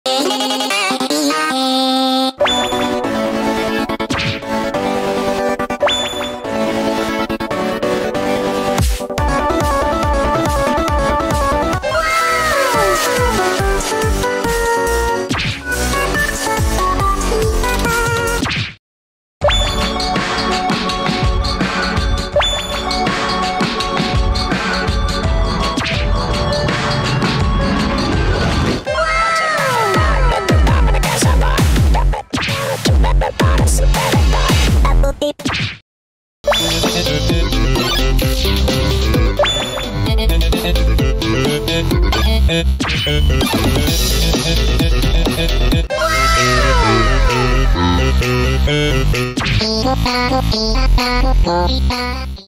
RWBY ▶ Blake Belladonna ➖ Clothes = ❓❓❓ | Art Memes ANIMATION
Hello everyone! New video for you! In this RWBY animation i did an experiment with Blake Belladonna and Clothes. I think this art memes compilation looks interesting.